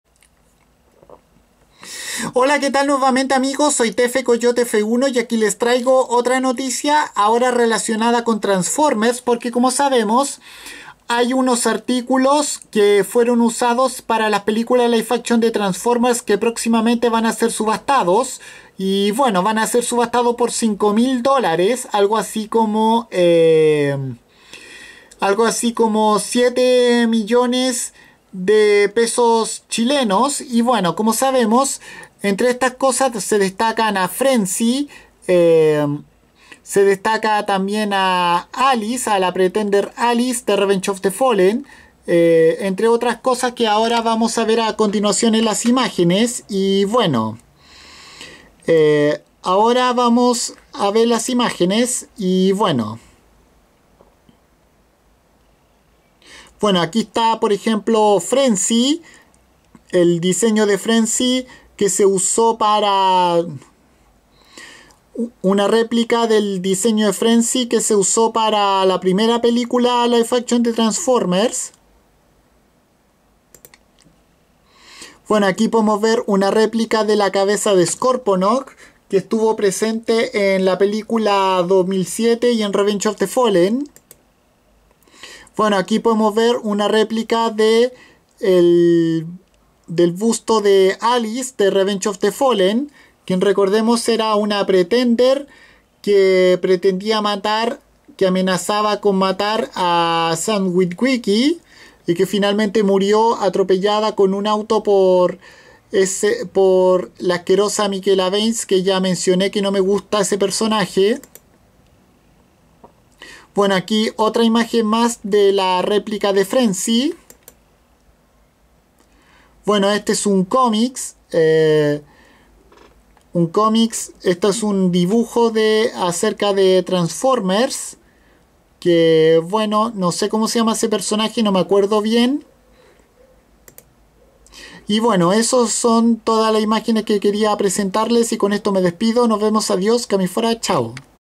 Algunas piezas de las películas live action de Transformers a subasta
En este vídeo les muestro réplicas de algunas piezas utilizadas para las filmaciones de las películas live action de la franquicia Transformers que se pondrán a subasta por $5000 dólares